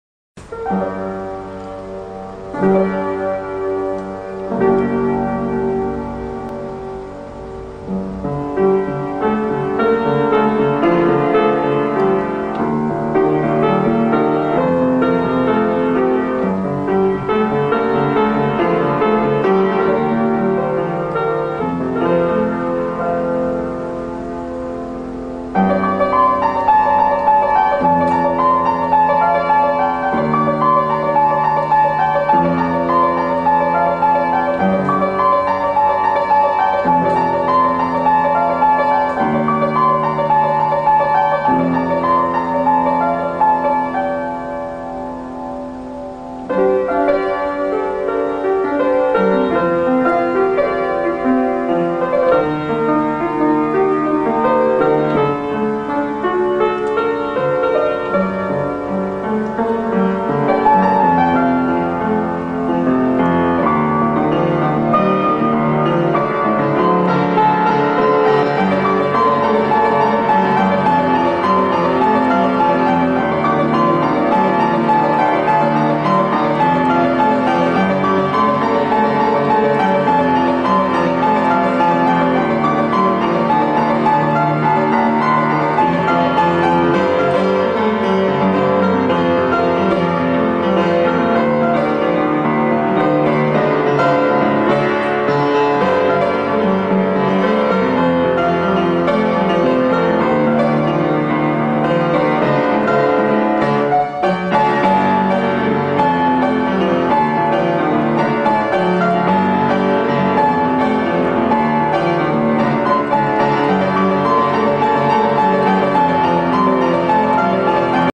San Leo
La pieve goticheggiante di San Leo in una rivisitazione adeguata a far risaltare la patria di grandi alchimisti. La musica di sottofondo la suono io, e mi son ispirato ad un evento della mia infanzia